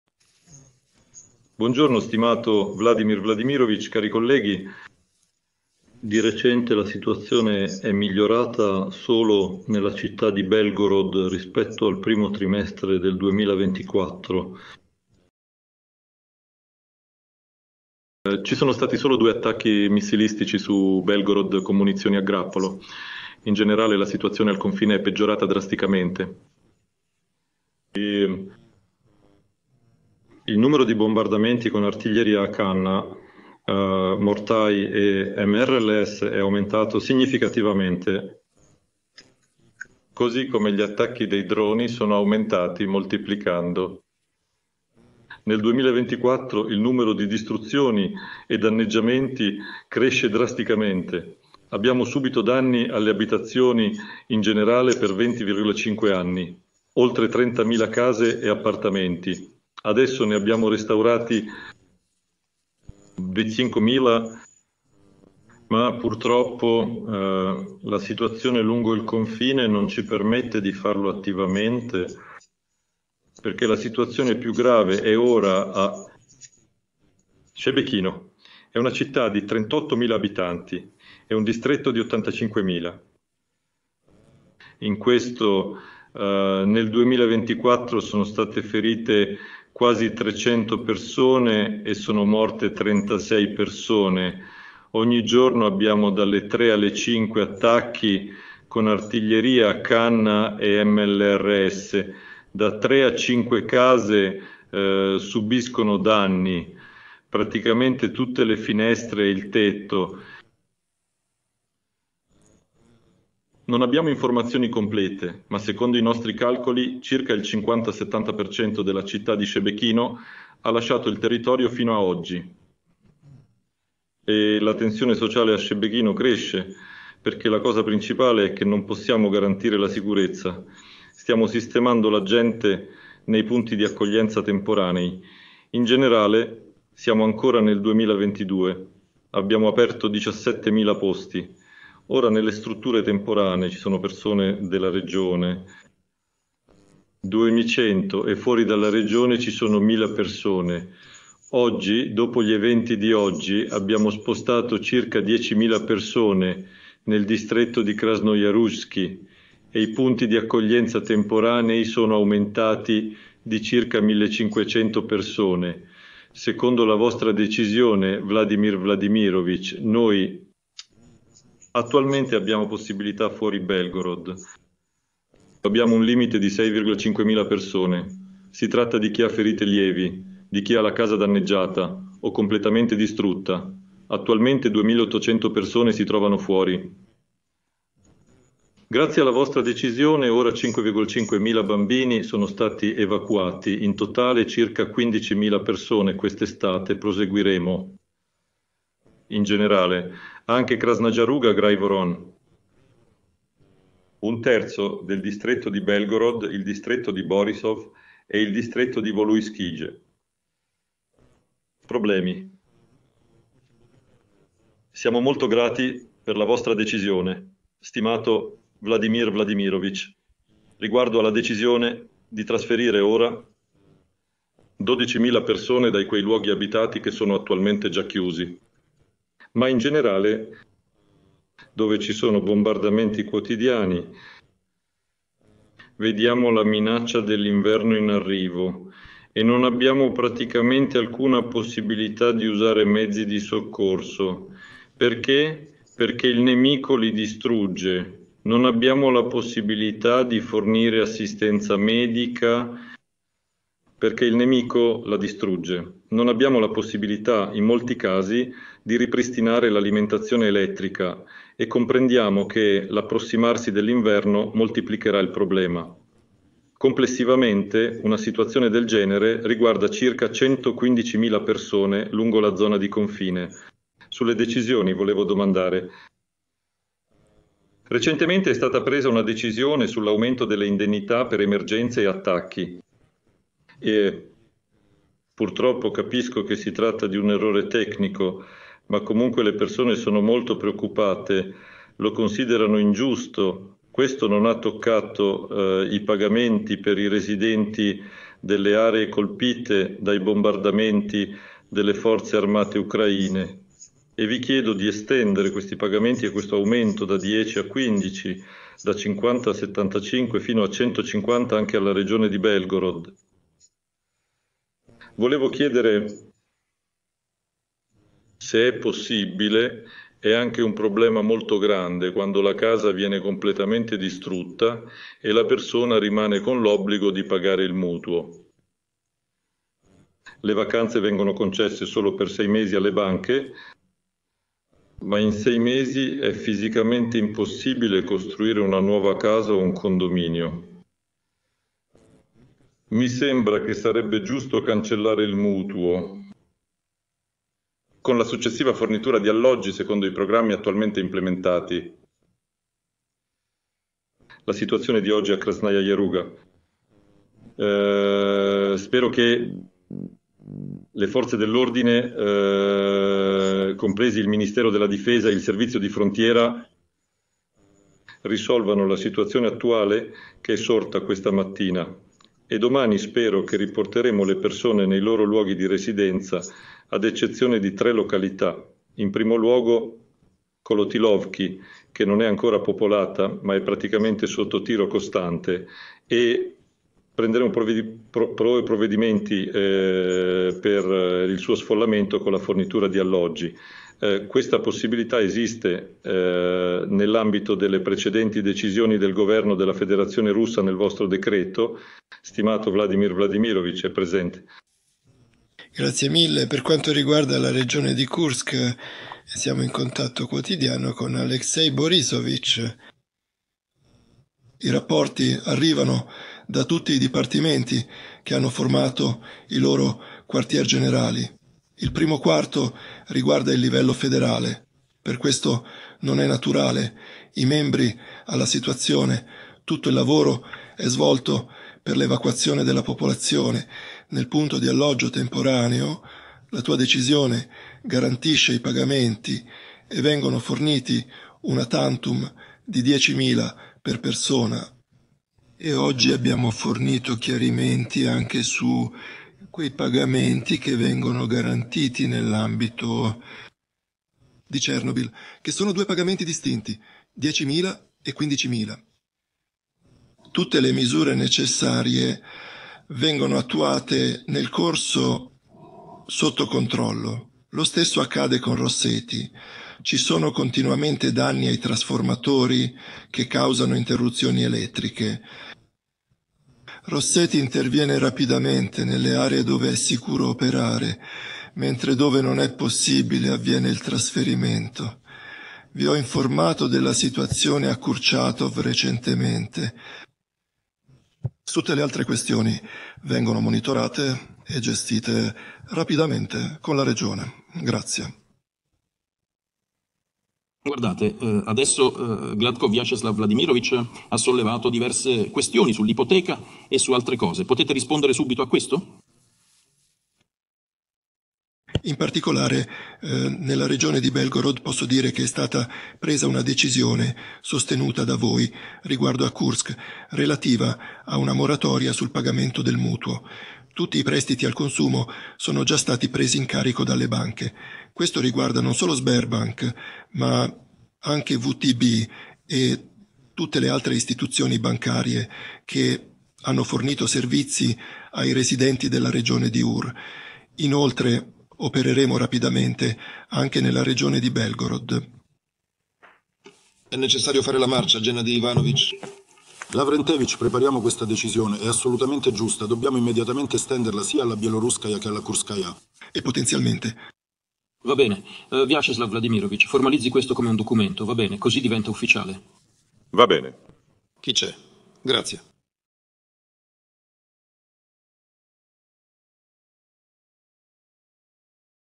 Situazione Critica nella Regione di Belgorod Valerij Gladkov Informa Putin
Valerij Gladkov ha informato Vladimir Putin riguardo al rapido peggioramento della situazione nella regione di Belgorod. Scopri i dettagli sulle difficoltà che la regione sta affrontando e le misure prese dal governo russo per fronteggiare la crisi. Guarda il video per approfondire gli sviluppi recenti e le risposte del governo. La situazione nelle aree di confine della regione di Belgorod è peggiorata drasticamente negli ultimi giorni. Il governatore della regione, Vyacheslav Gladkov, ha riferito di questo al presidente russo Vladimir Putin durante una riunione tenutasi lunedì.
Secondo Gladkov, negli ultimi 2,5 anni, oltre 30.000 case e appartamenti sono stati danneggiati a causa dei bombardamenti. Sebbene 25.000 di essi siano già stati riparati, la situazione lungo il confine impedisce ulteriori lavori di restauro.
"In generale, la situazione lungo il confine è peggiorata drasticamente e il numero di bombardamenti è aumentato significativamente, sia con artiglieria convenzionale, mortai, sistemi di razzi a lancio multiplo (RSZO), sia con un incremento notevole degli attacchi da parte di droni", ha dichiarato Gladkov.

Putin ha tenuto la riunione nella sua residenza a Novo-Ogarevo, alla quale hanno partecipato membri del Consiglio di Sicurezza, del governo, rappresentanti dell'amministrazione presidenziale, capi delle forze di sicurezza, e i governatori delle regioni di Belgorod, Bryansk e Kursk.

Nella regione di Kursk, i combattimenti con le forze armate ucraine sono in corso dal 6 agosto. È stato dichiarato lo stato di emergenza di livello federale. Il 10 agosto è stato introdotto anche un regime di operazioni antiterrorismo (KTO) nelle regioni di Kursk, Belgorod e Bryansk.

LIBRO CONSIGLIATO